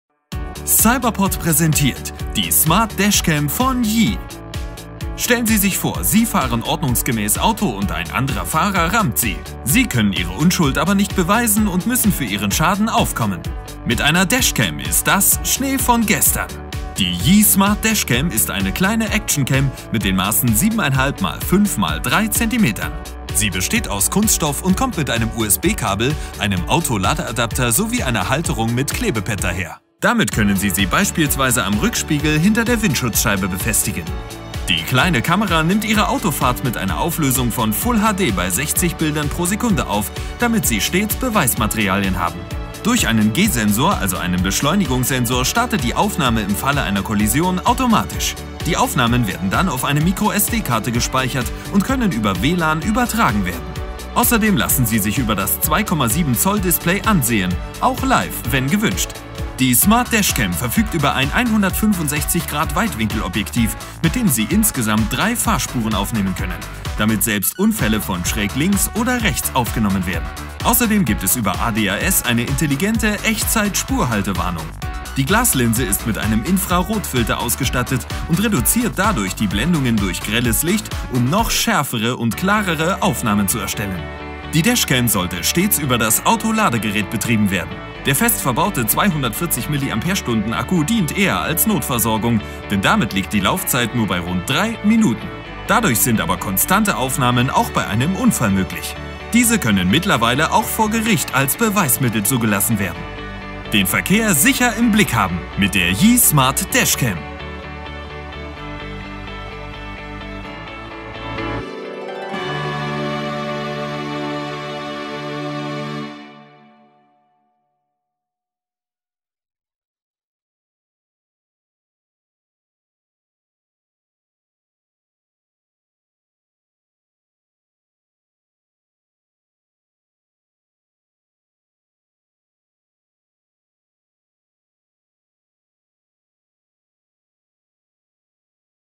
YI Smart Dash Cam Shortcut I Cyberport
Ihr fahrt ordnungsgemäß Auto und jemand rammt euch. Ihr könnt eure Unschuld aber nicht beweisen? Mit der Smart Dash-Cam von YI sind solche Szenarien Schnee von gestern. Wir stellen euch die kleine Actioncam mal vor.

Unsere Links!

Werdet Fan unserer Facebook-Seite!

Besucht uns im Store!

Für weitere Anfragen:
cyberport(at)googleplus.de